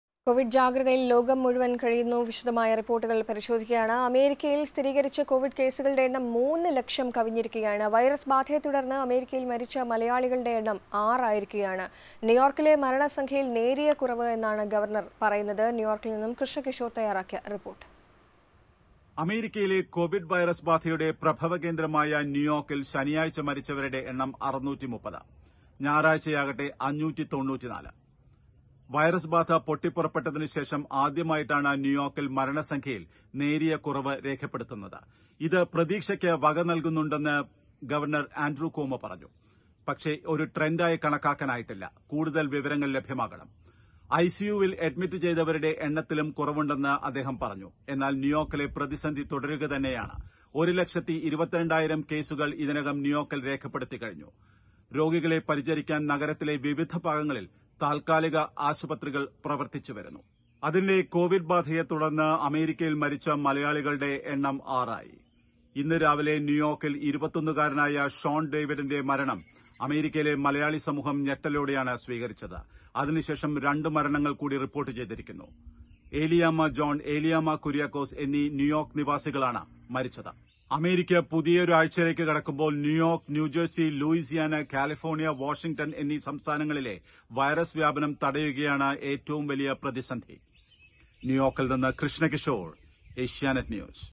അമേരിക്കയിൽ കൊവിഡ് ബാധിതരുടെ എണ്ണം 3 ലക്ഷം കവിഞ്ഞു | NewYork Updates
More than 3 Lakh covid patienst in America